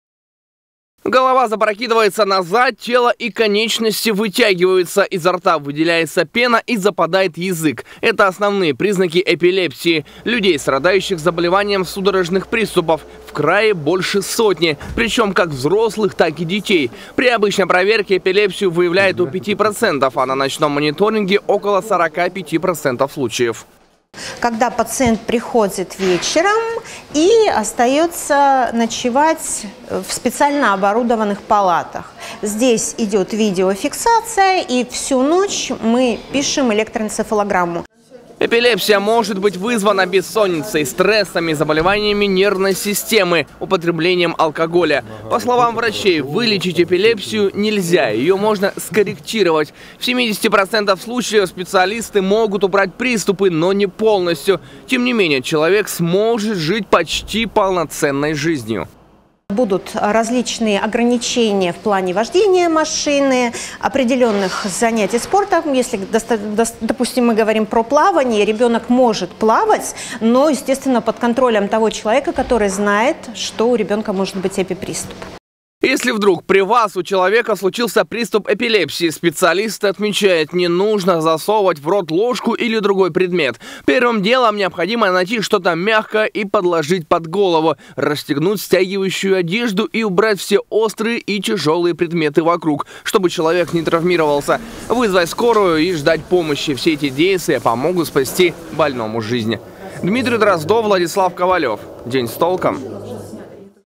Как помочь больному эпилепсией и можно ли вылечить недуг
Фиолетовый день и как он связан с эпилепсией. Его придумали, чтобы поддержать людей, страдающих заболеванием судорожных приступов. Таких в Алтайском крае больше сотни. Как правильно помочь человеку с эпилепсией и можно ли ее вылечить — рассказываем в сюжете.